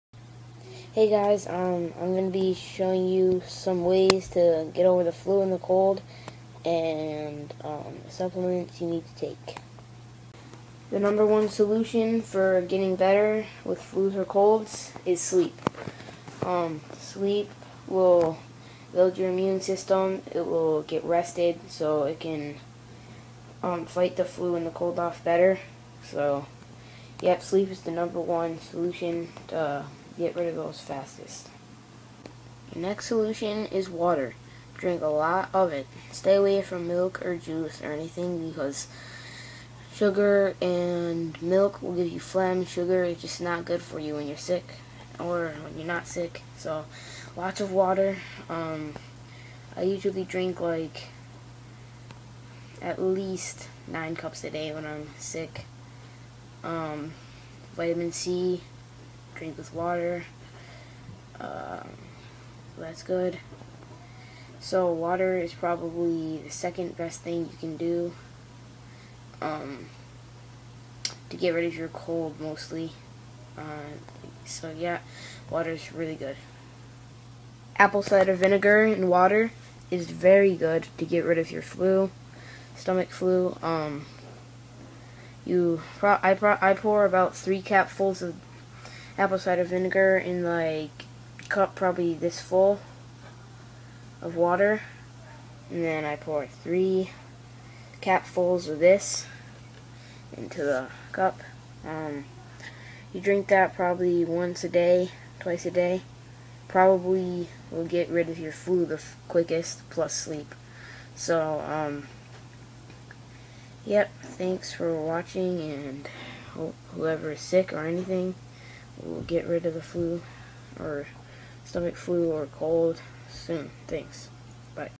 Quickest ways to get rid of the flu or cold
Sick,sleep,drink,other. This may not work it has worked for me but may not for u just a heads up.